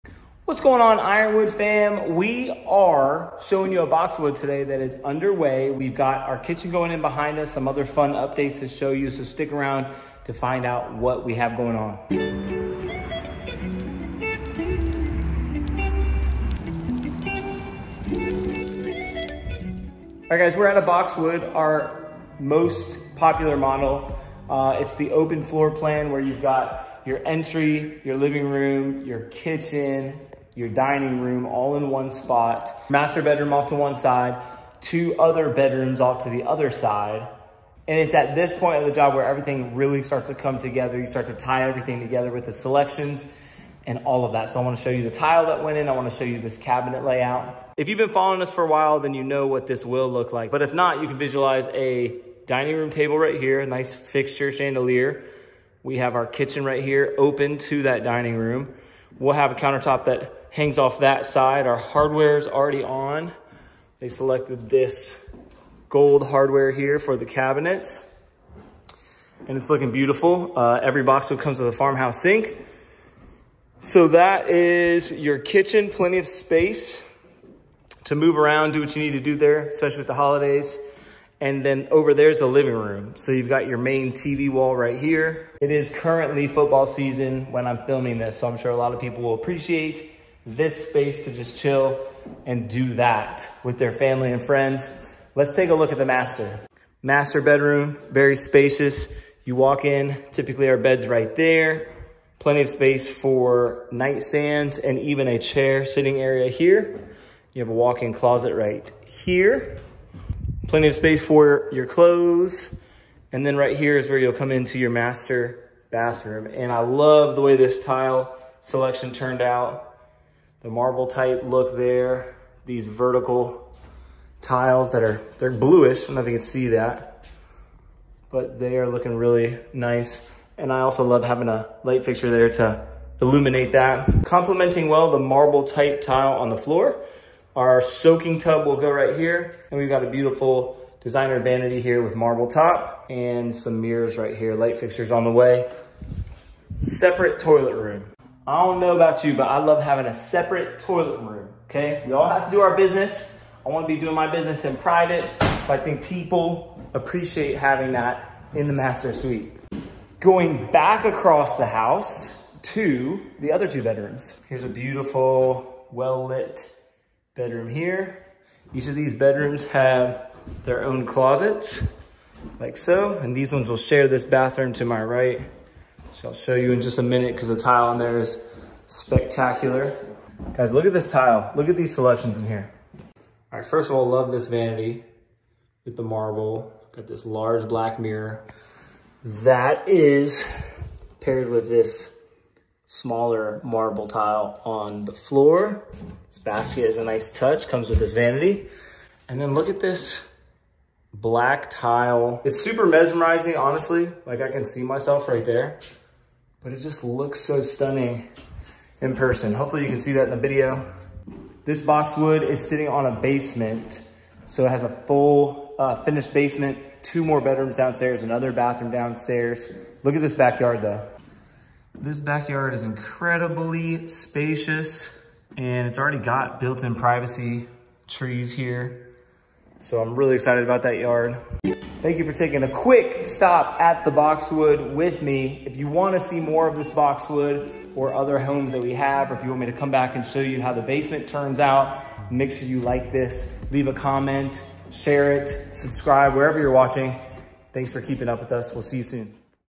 Boxwood Tour: New Home in Fredericksburg Virginia
Welcome to a stunning new construction home tour in Fredericksburg, VA by Ironwood Homes! Explore the latest in modern design and luxury living as we take you through this beautifully crafted masterpiece. From spacious interiors to state-of-the-art features, you won't want to miss this exclusive look. Join us in discovering your dream home today!

¨¨¨¨¨¨¨¨¨¨¨¨ SOME OF OUR FAVORITE VIDEOS ¨¨¨¨¨¨¨¨¨¨¨¨